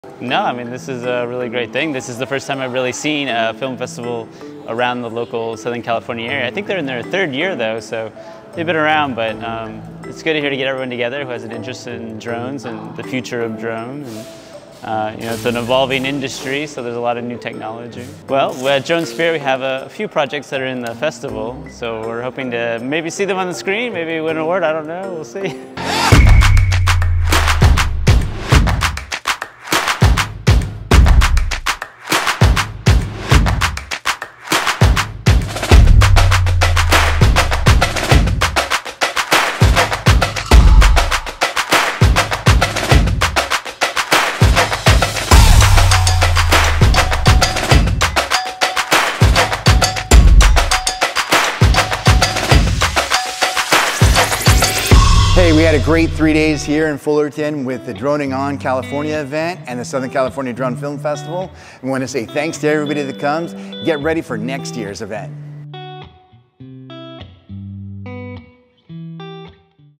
Interview with Nathan D. Snyder & Natalya Snyder | Dronespeare | SoCal Drone Film Festival 2023
Red carpet sound bytes from Nathan D. Snyder and Natalya Snyder from Dronespeare at the 2023 SoCal Drone Film Festival, on July 22nd in Fullerton, CA.

Dronespeare's video "Destination Hawai'i: O'ahu" was awarded Best News / Documentary at this year's festival.

Filmed and edited by @ZawStudiosInc